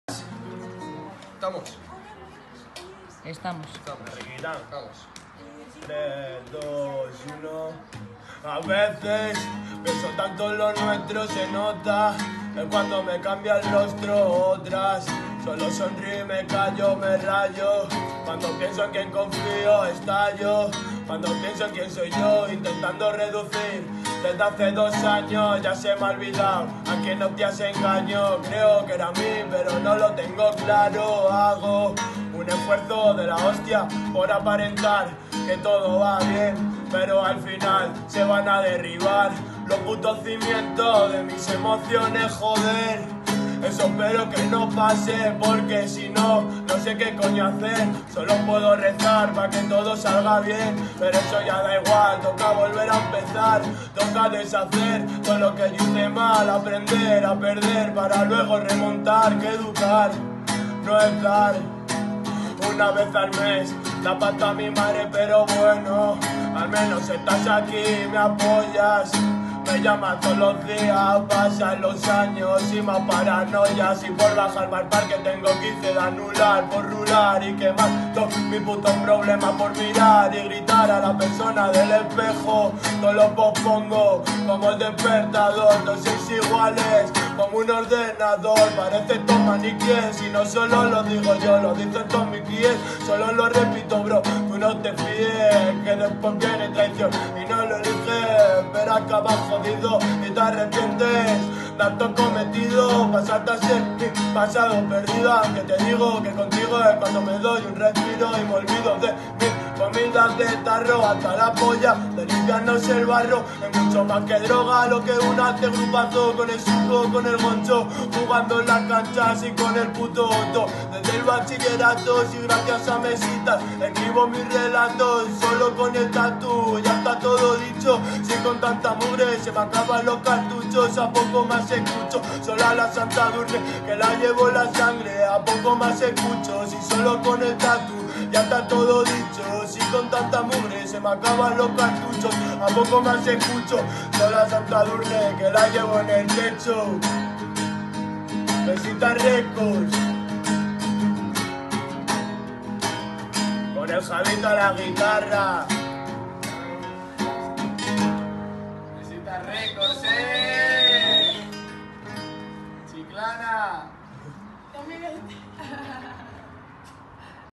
MIRALLES X JAVI ROMERO - ONE SHOT
Un one shot guapo guapo pa estos dias de veranito. MSTS ON TOUR. LOS CAZURROS TODOS DE LA CABEZA.

A la guitarra: @druvijavi